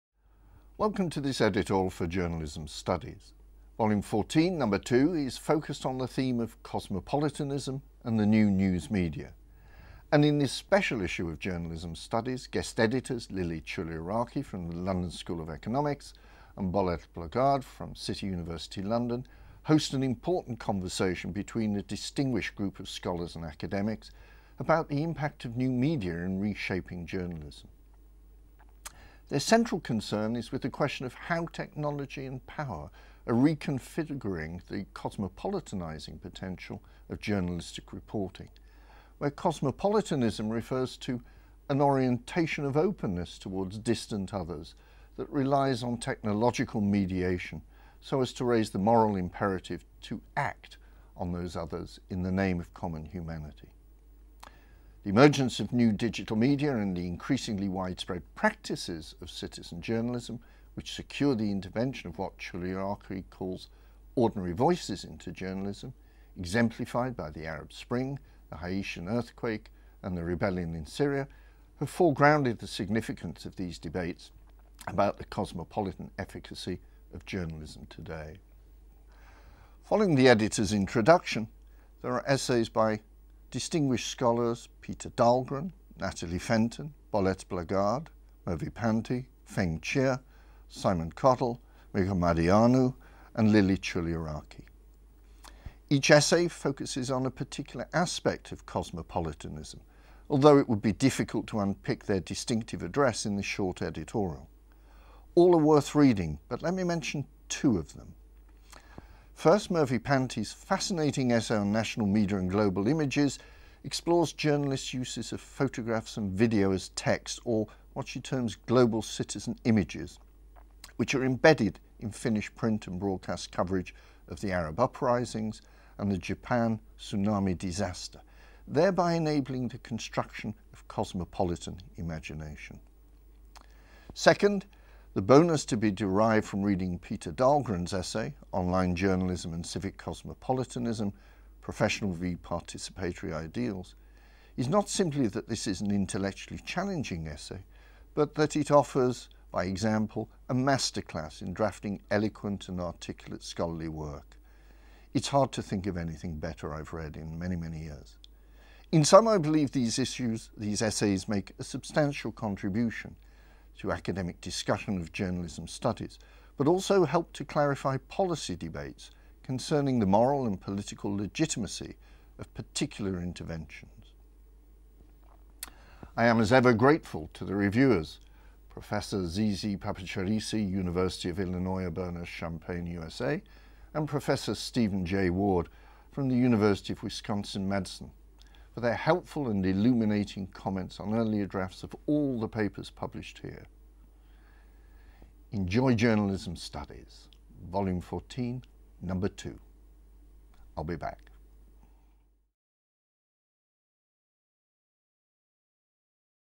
Journalism Studies April 2013
April's Special issue of Journalism Studies 'Cosmopolitanism and the New News Media' is edited by Lilie Chouliaraki and Bolette Blaagaard

Contents
Online Journalism and Civic Cosmopolitanism: Professional vs. participatory ideals
Peter Dahlgren

Cosmopolitanism as Conformity and Contestation: The mainstream press and radical politics
Natalie Fenton

Situated, Embodied and Political: Expressions of citizen journalism
Bolette Blaagaard

Getting Closer? Encounters of the national media with global images
Mervi Pantti

The world is watching: The mediatic structure of cosmopolitanism
Pheng Cheah

Journalists Witnessing Disaster: From the calculus of death to the injunction to care
Simon Cottle

Remediation, Inter-mediation, Transmediation: The cosmopolitan trajectories of convergent journalism
Lilie Chouliaraki